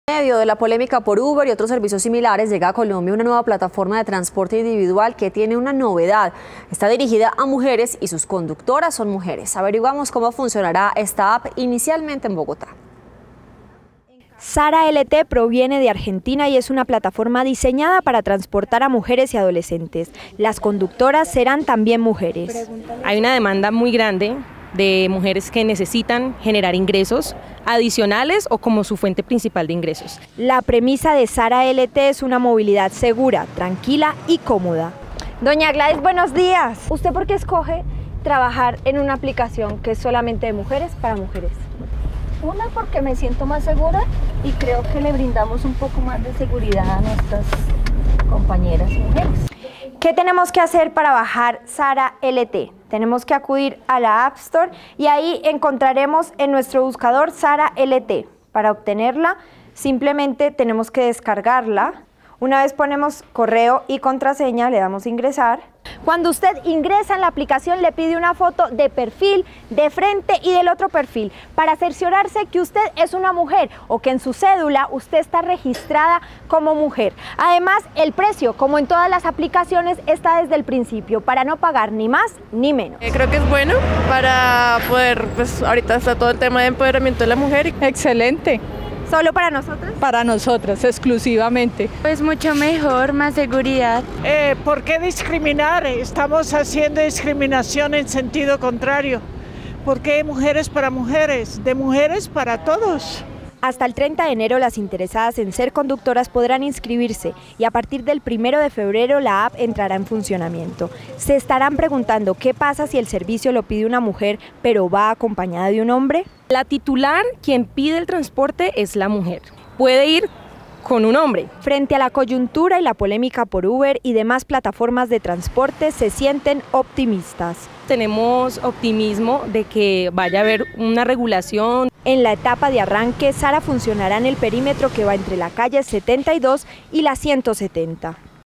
En medio de polémica por Uber, llega a Colombia app de transporte dirigida a mujeres
Se llama Sara LT y los vehículos son manejados únicamente por conductoras. Entrará en funcionamiento el primero de febrero.